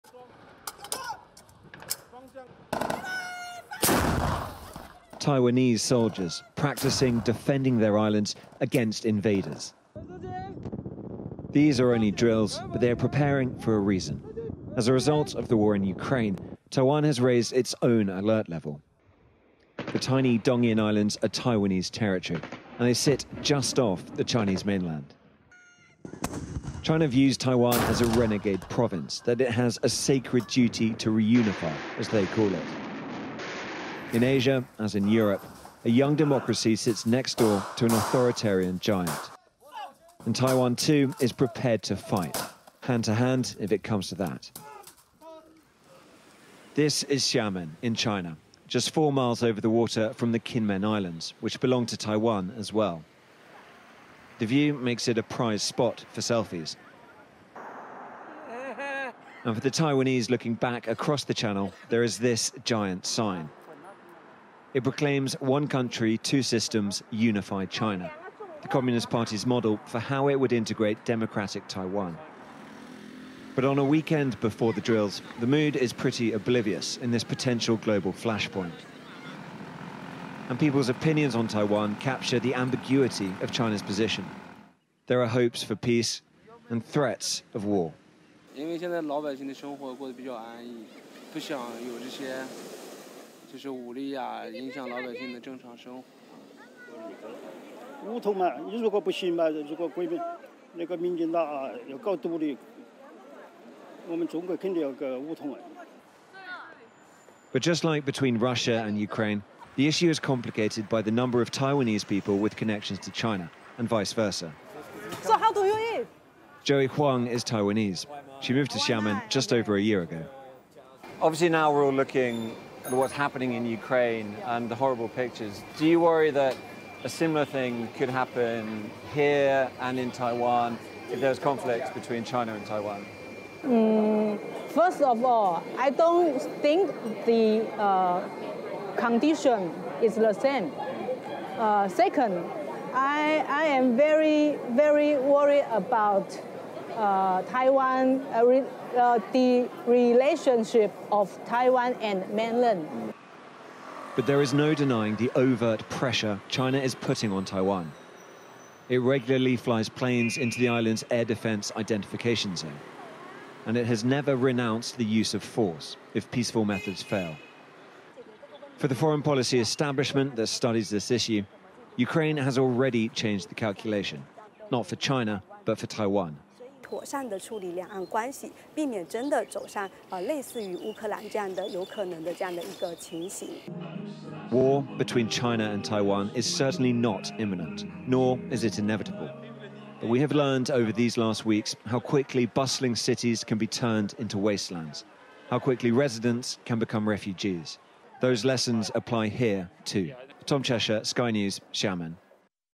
Taiwan stages military drills - but they don't want war
Taiwan has staged a series of military drills but the people there are not interested in war.

Sky's Asia correspondent Tom Cheshire reports from the city of Xiamen.